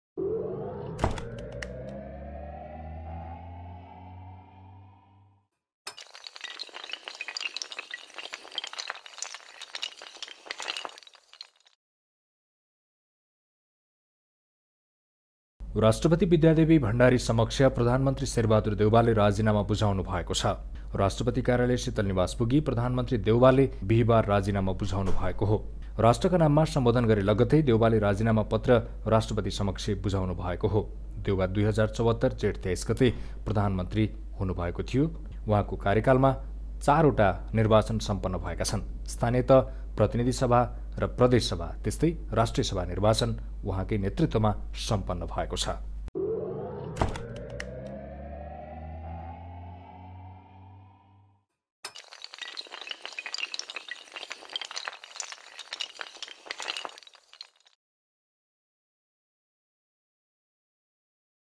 प्रधानमन्त्री देउवाकाे राजीनामा
प्रधानमन्त्री शेरबहादुर देउवाले बिहीबार राजीनामा दिएका छन् ।